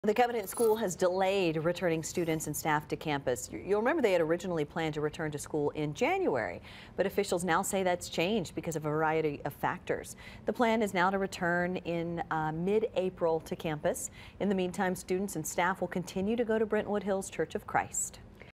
Covenant School delays return to campus
School officials originally planned to return to campus in January.